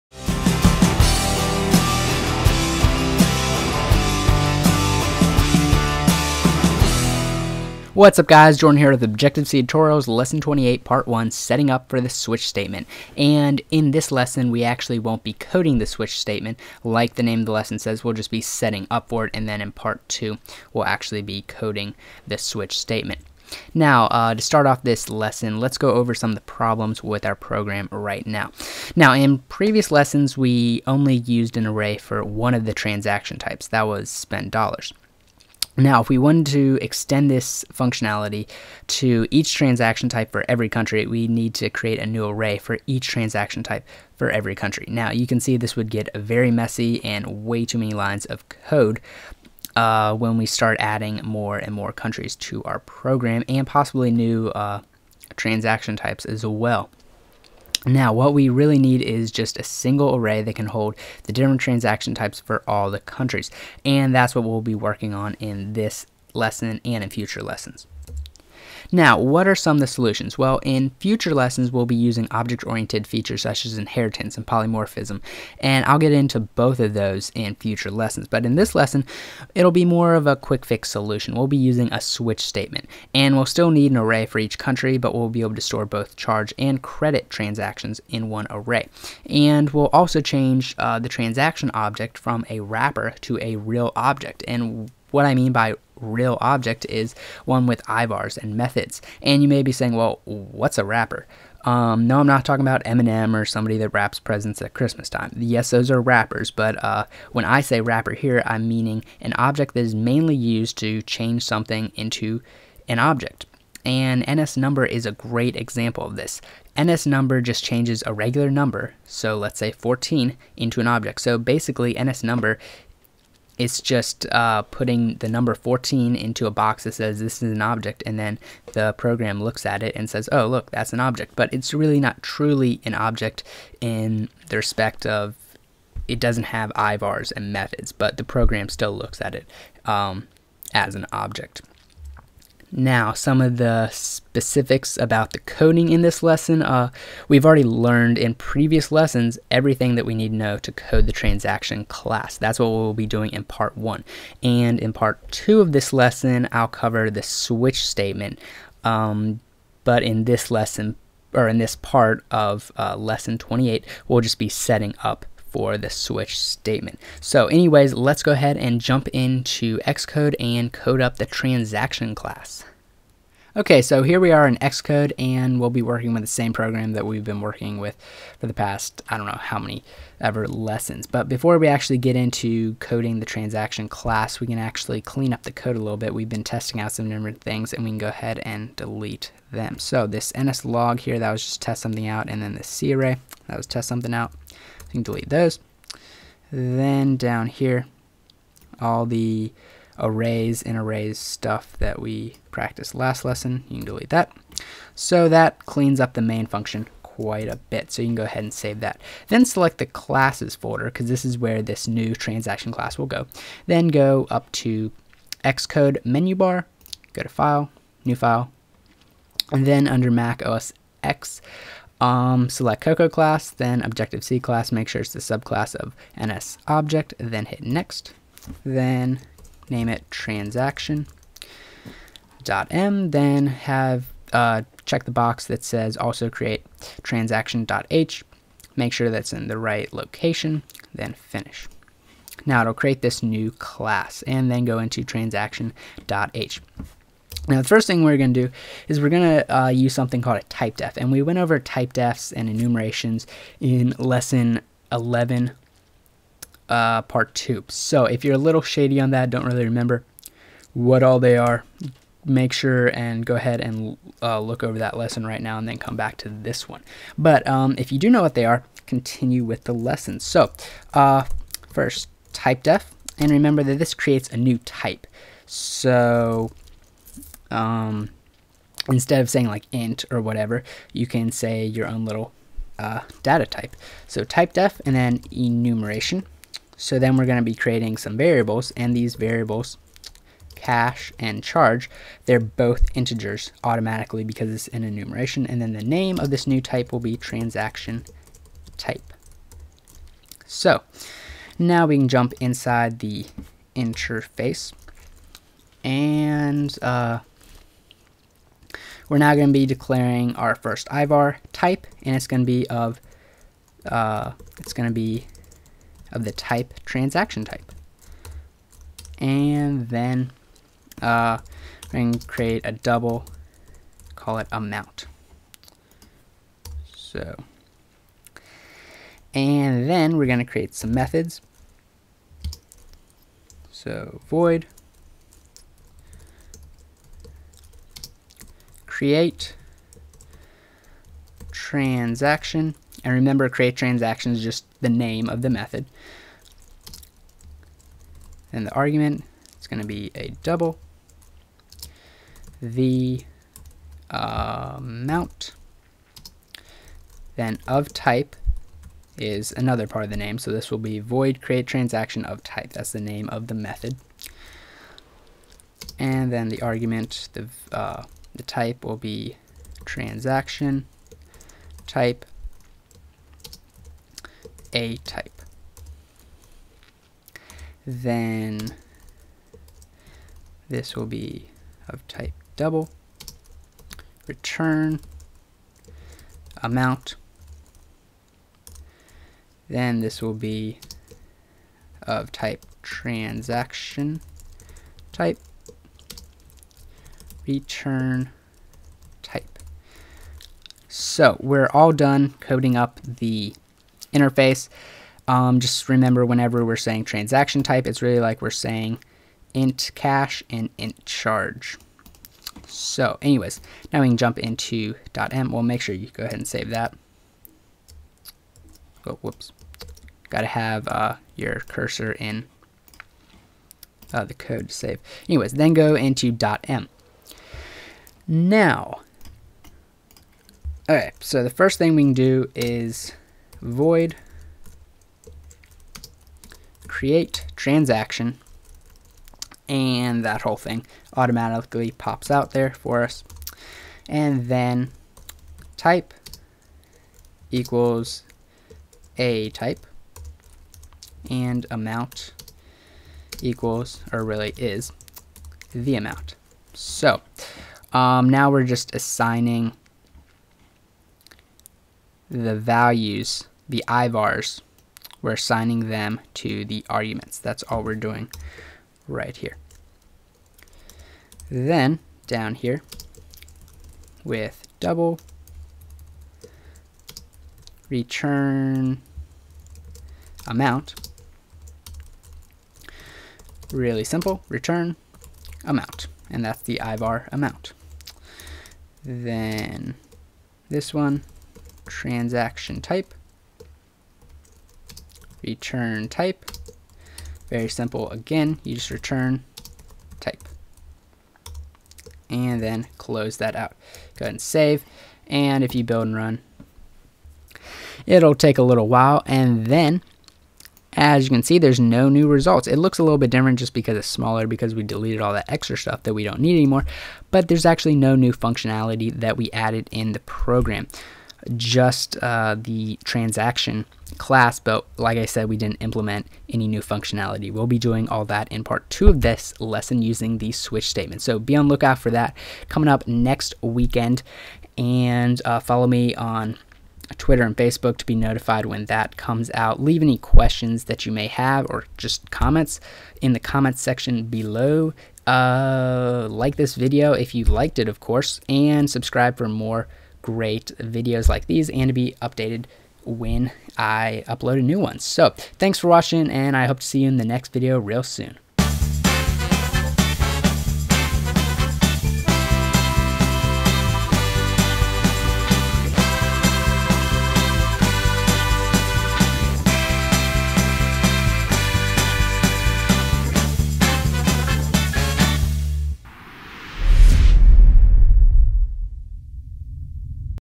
Objective-C Tutorial - Lesson 28: Part 1: Setting Up for the Switch Statement
In this lesson I go over some of the problems with our program and how we can fix them.  One of the quick fixes is with the switch statement.  We will be setting up for that in this lesson (part 1) and coding it in the next (part 2).

objective-c tutorial
objective-c tutorials
Objective-C Tutorial
objective c programming tutorial
objective c programming tutorials